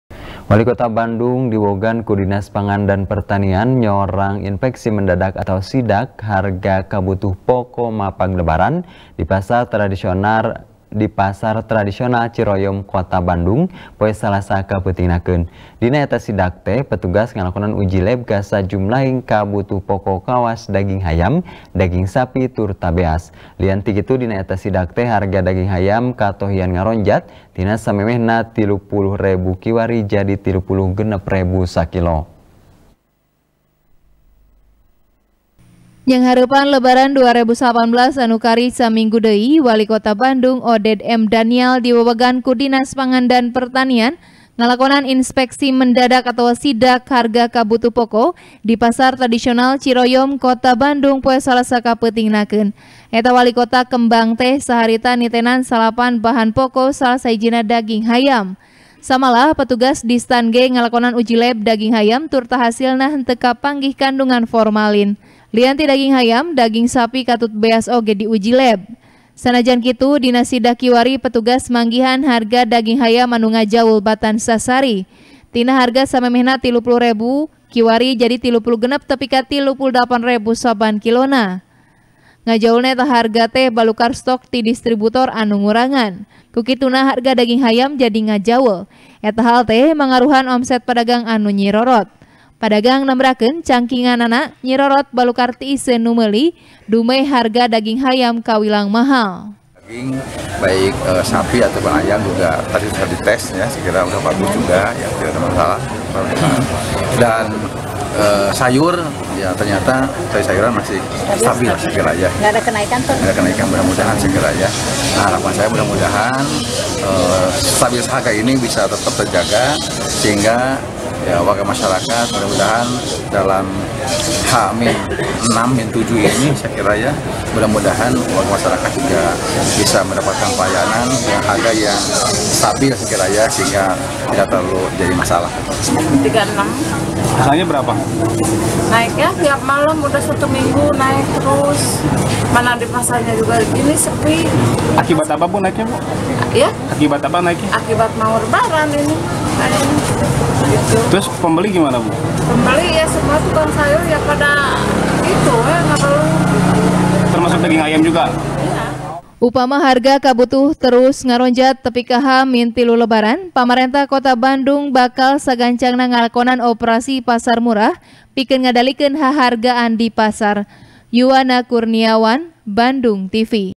Mapag Lebaran, pemkot Bandung nyorang sidak tur uji lab kabutuh poko | TP | BANDUNGTV
TP | BANDUNGTV | IWAN M RIDWAN | Nyanghareupan Lebaran 2019 anu kari saminggu deui, walikota Bandung Oded M. Danial, diwewegan ku dinas pangan dan pertanian, ngalakonan inpeksi mendadak atawa sidak harga kabutuh poko, di pasar tradisional Ciroyom kota Bandung, poe Salasa kapeutingnakeun. Eta walikota kembang teh, saharita nitenan 9 bahan poko salah sahijina daging hayam. Samalah, petugas distan ge ngalakonan ujilab daging hayam tur hasilna teu kapanggih kandungan formalin. Lian ti daging hayam,  daging sapi katut beas oge diuji lab.